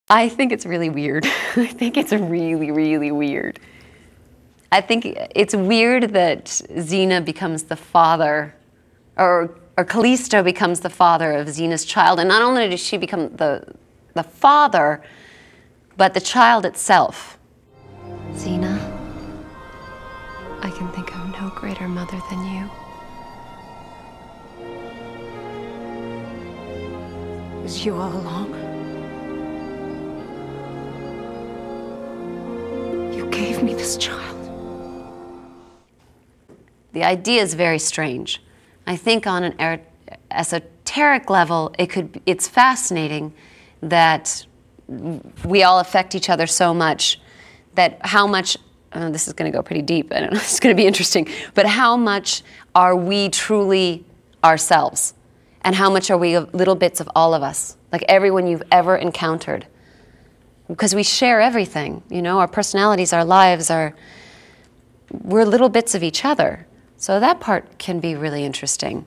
Seeds of Faith DVD Interview with Hudson Leick
Hudson talking about her experiences filming "Seeds of Faith"

I DO NOT OWN XENA NOR DO I REPRESENT HUDSON LEICK.